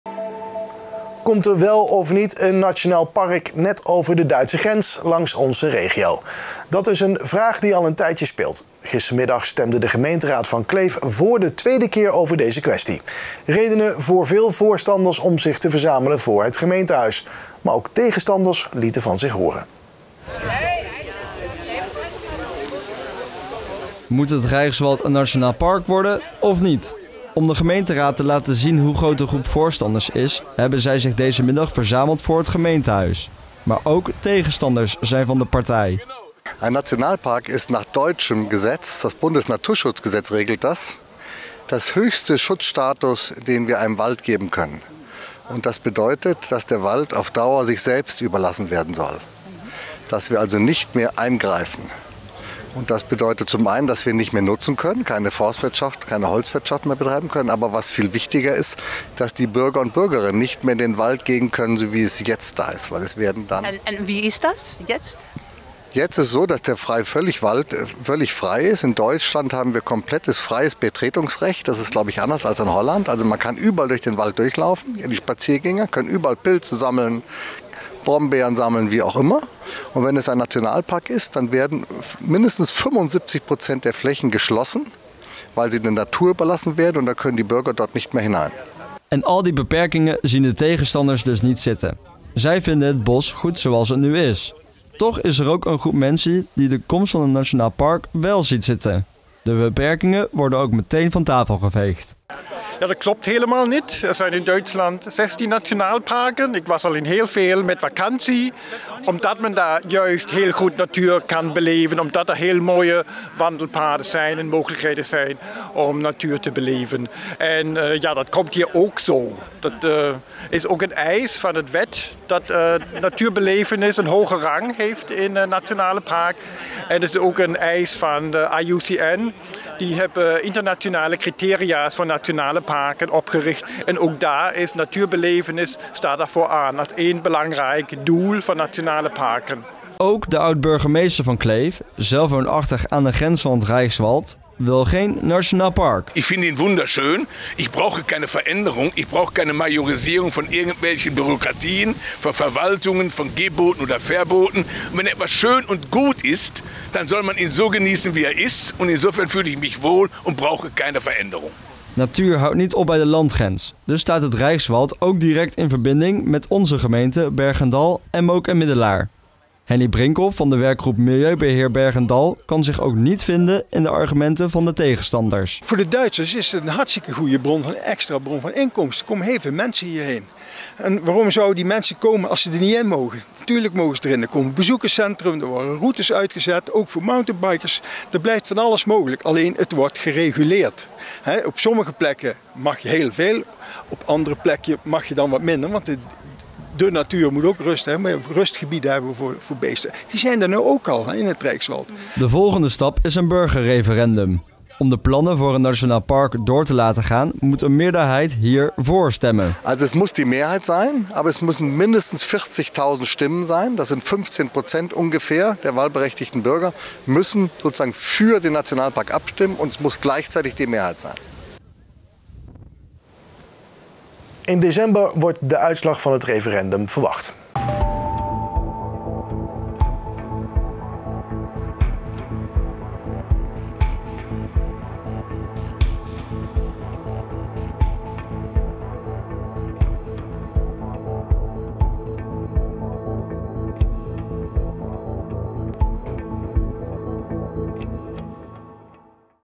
Kleve stemt tegen nationaal park, referendum op komst || RN7 REGIONIEUWS
Komt er wel of niet een nationaal park, net over de Duitse grens, langs onze regio? Dat is een vraag die al een tijdje speelt. Gistermiddag stemde de gemeenteraad van Kleef voor de tweede keer over deze kwestie. Reden voor veel voorstanders om zich te verzamelen voor het gemeentehuis. Maar ook tegenstanders lieten van zich horen.